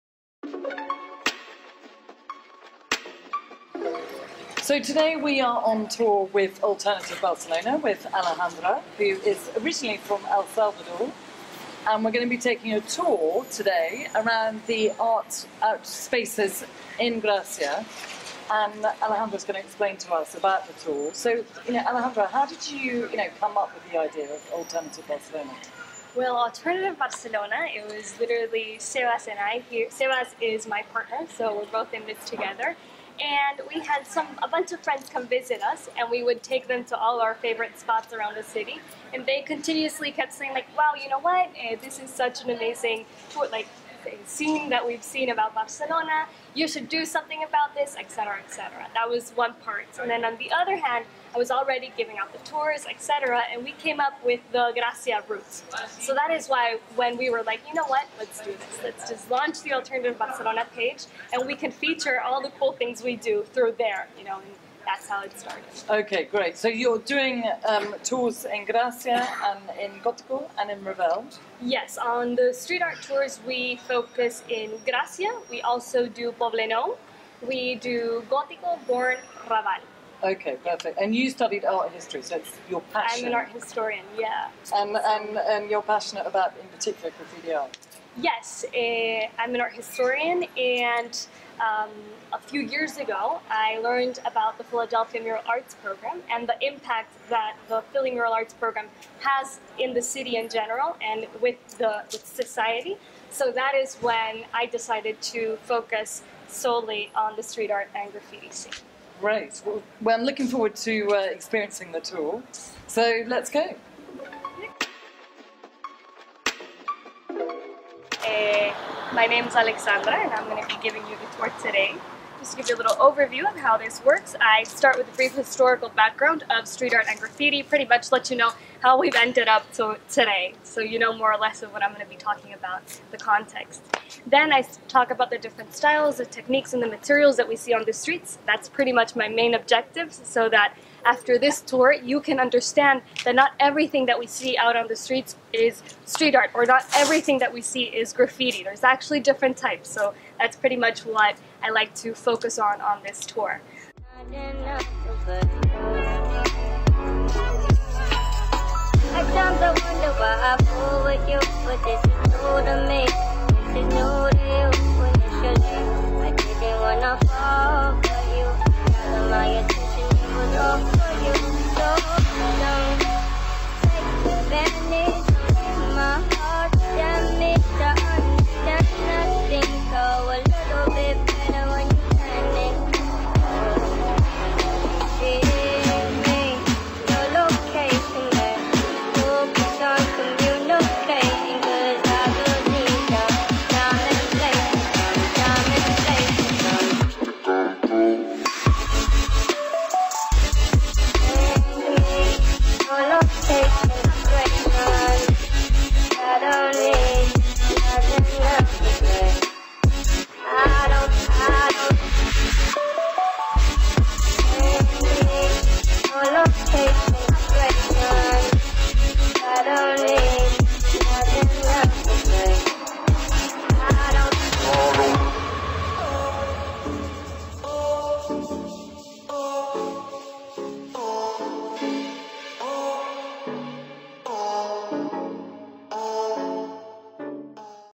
People On The Grid with Alternative Barcelona
A week ago, we experienced a graffiti tour with Alternative Barcelona in Gracia. We were able to see incredible pieces of art! Check out their Facebook page if you would also like to experience that tour.

Credits:
~ Khalid: